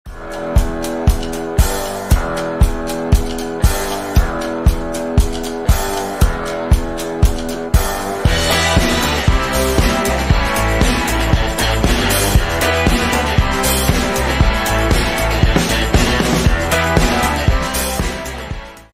Walk Through Tilbury Auto Sales RV Yamaha
Walk through of our body shop and service centre!
Tilbury Auto Sales RV & Yamaha
We service everything!